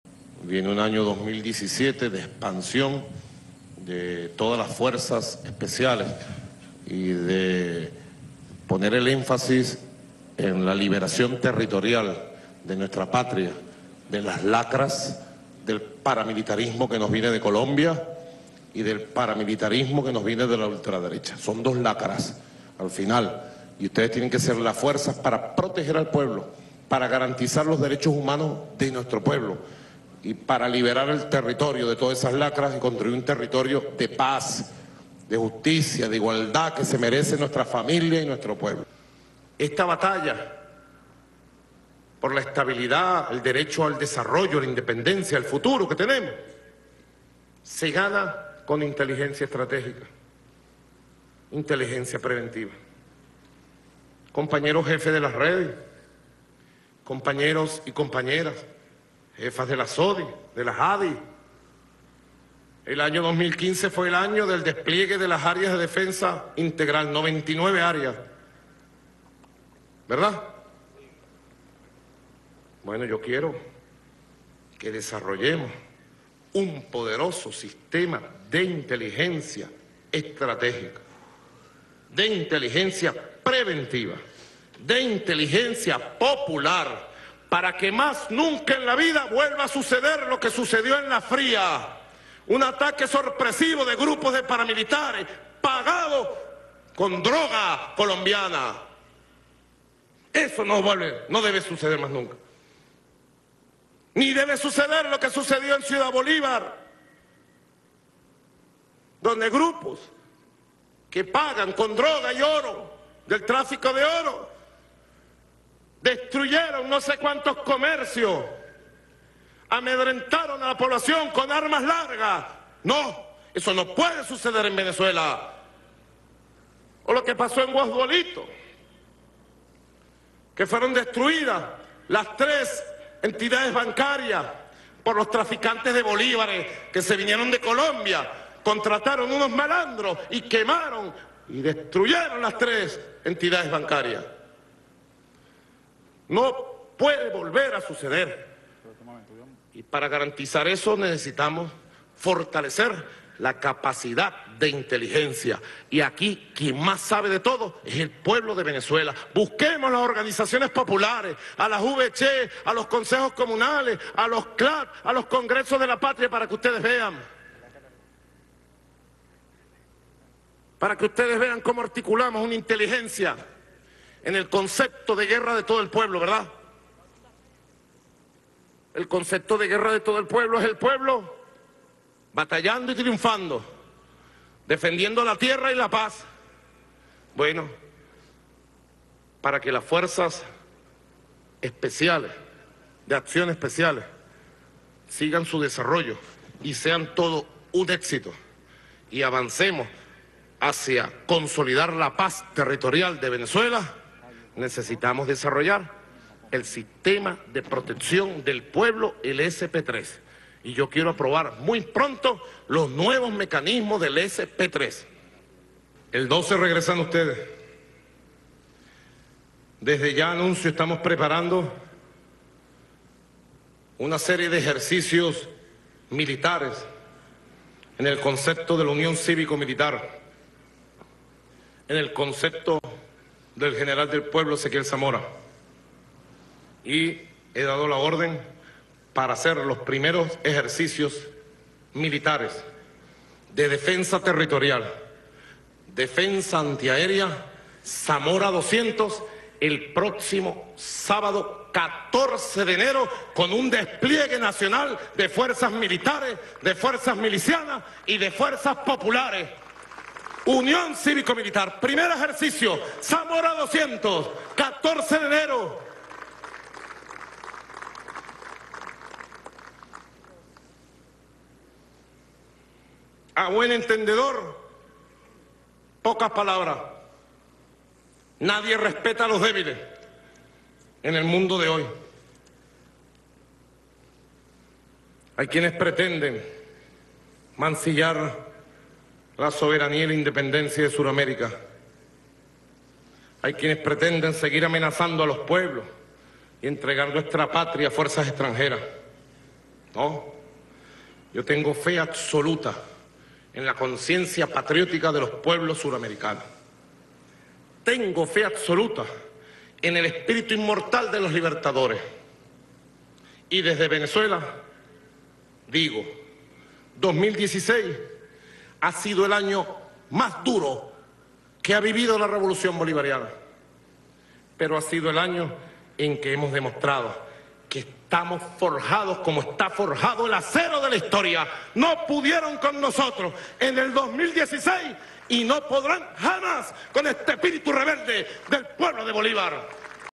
Maduro llama a FANB a acciones de inteligencia para repeler ataques paramilitares
Campo de Carabobo, 28 de diciembre de 2016. Diferentes momentos del discurso de Nicolás Maduro este 28 de diciembre de 2016, en el que llama a las Fuerzas Armadas a desarrollar acciones para luchar contra el paramilitarismo colombiano y de ultraderecha, recordando las recientes acciones en La Fría (destrucción de la alcaldía), Guasdualito (destrucción de 3 agencias bancarias) y el estado Bolívar (saqueos). Insta a sus soldados a poner el énfasis en la liberación territorial de las  lacras del paramilitarismo colombiano y el paramilitarismo de la ultraderecha venezolana. Convoca a ejercicios cívico-militares Zamora-200 el 14 de enero de 2017.